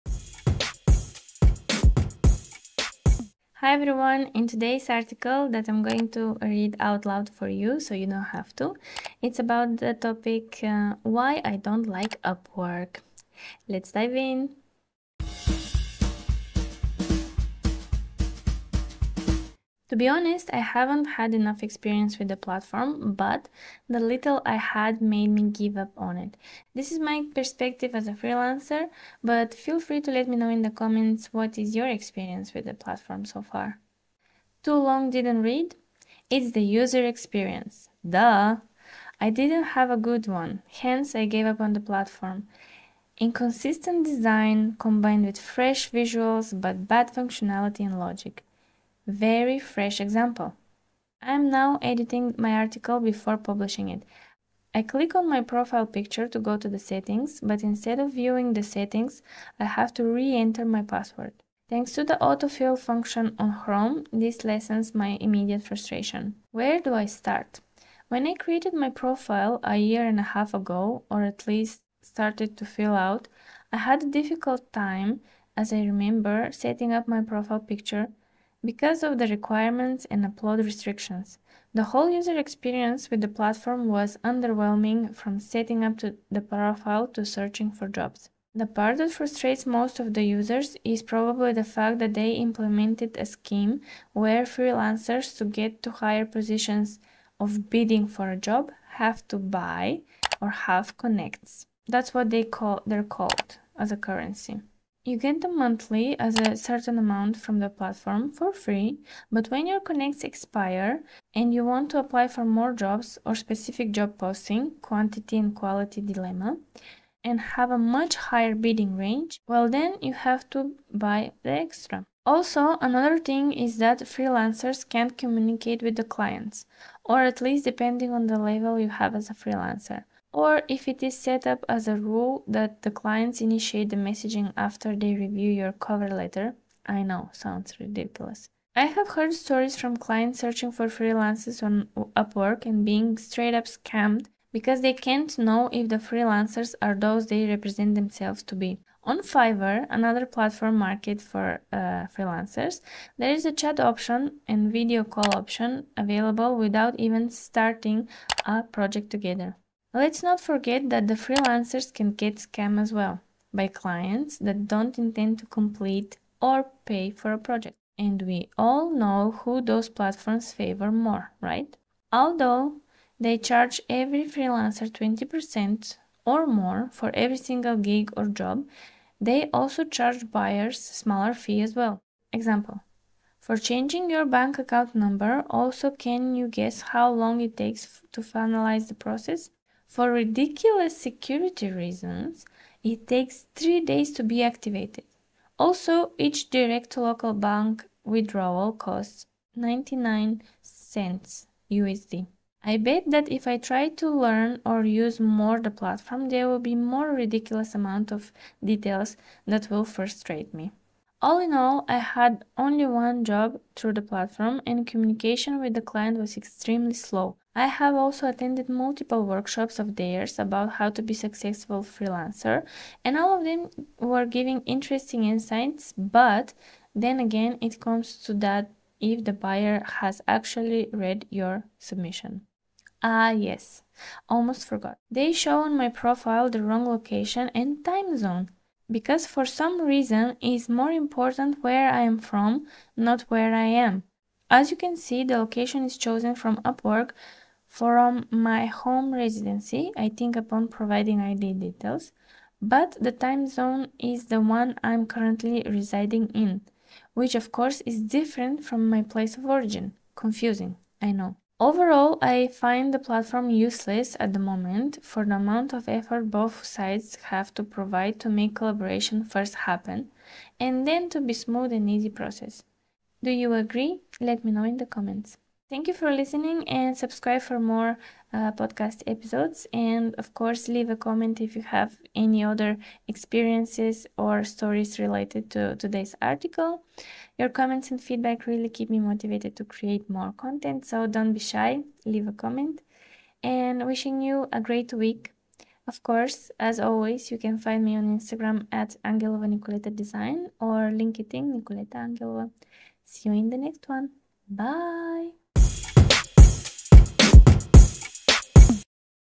Why Upwork has a Terrible User Experience for Freelancers? Is It Worth Your Time and Effort?
Hey there, it's me Nikoleta 😁 and in this video, I want to share with you my personal experience as a freelancer on Upwork. As you probably know, Upwork is one of the most popular online marketplaces for freelancers to find work and for clients to hire them. However, my experience using Upwork was far from perfect. In fact, I found the user experience to be terrible.

In this video, I'll be reading one of my articles about Upwork where I go into detail about my frustrations with the platform. From the complicated sign-up process to the low-quality job postings, I'll be sharing all the reasons why I believe Upwork is a terrible user experience for freelancers and need to focus on the improvement of it.

If you're a freelancer or thinking about becoming one, you won't want to miss this video. Join me as I share my thoughts and experiences with Upwork and help you decide whether it's the right platform for you. Time is money, and you should invest it responsibly.

Chapters:
0:00 Intro
0:19 My experience with Upwork so far
0:48 Fresh example of the bad user experience
1:28 The frustrating connect system
2:01 Limited communication between clients and freelancers
3:31 Workshops, advice, hacks, but it's the numbers game really
3:52 The whole location mystery
4:21 Overall impression
4:35 Outro

👉🏽 P.S. You can support my journey by buying me a coffee